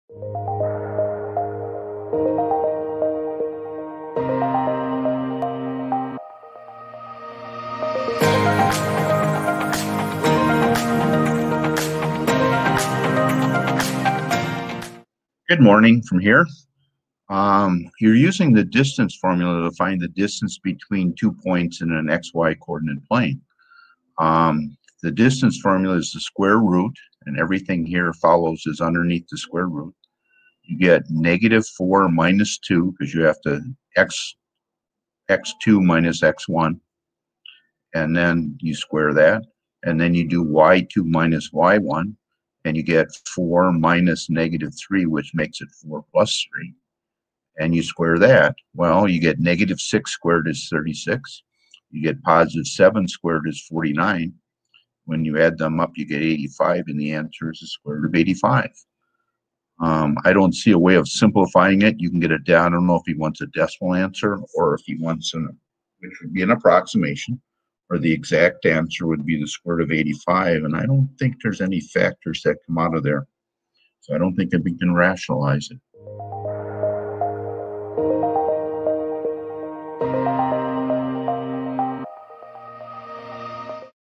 distance formula in x-y coordinate plane
Question: A circle has a diameter with endpoints A(-4,4) and B(2,-3). What is the length of the diameter

Answered By:

Dennis S.